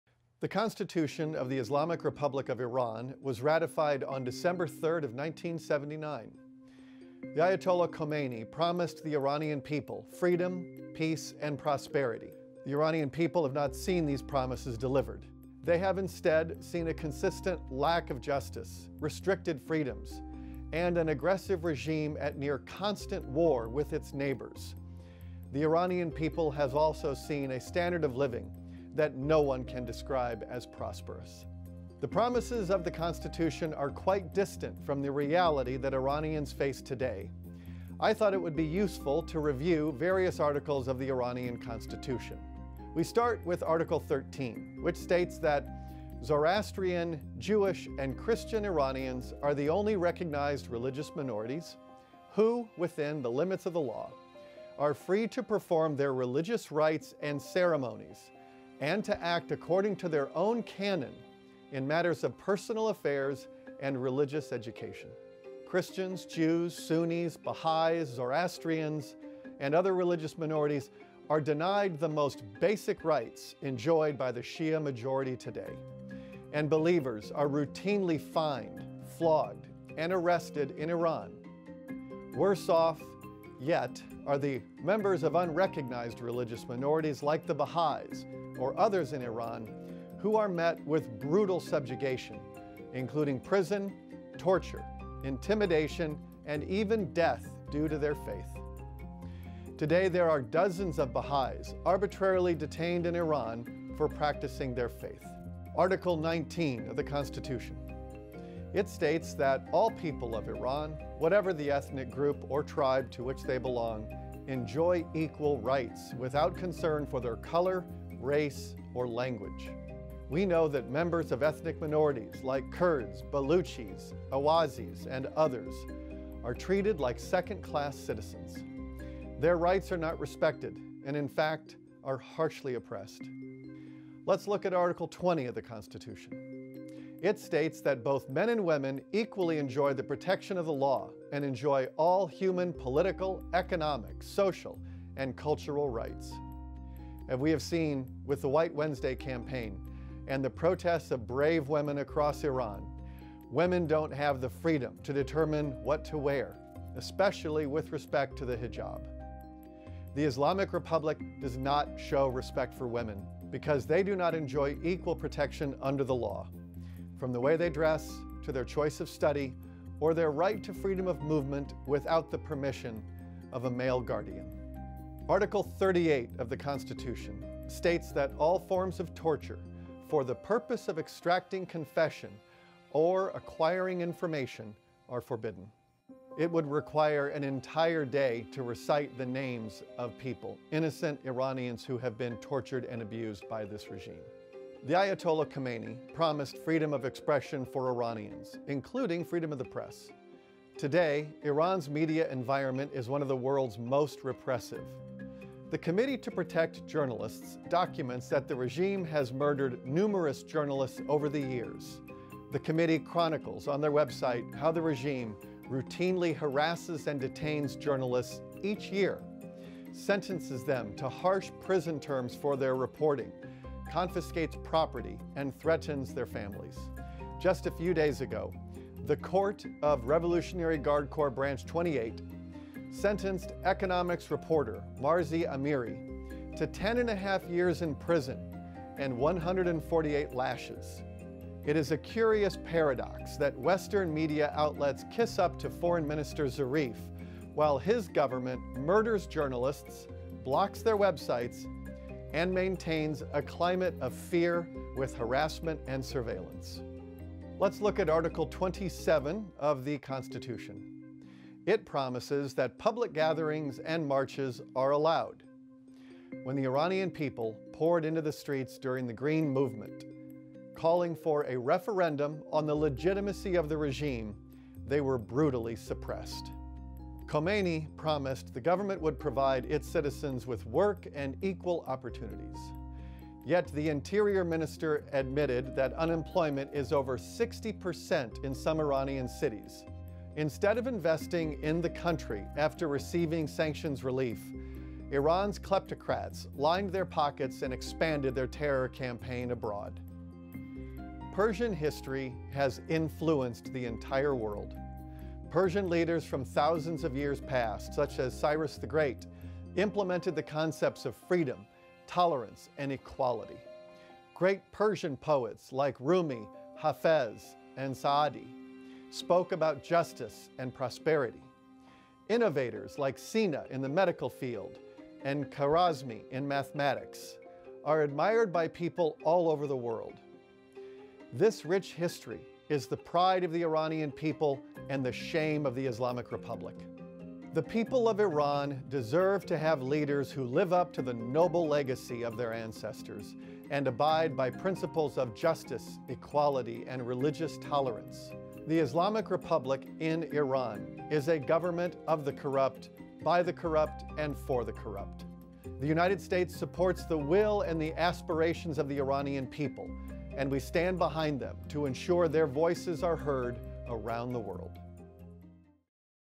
The Constitution of the Islamic Republic of Iran
Special Representative for Iran Brian Hook comments on the constitution of the Islamic Republic of Iran in September 2019.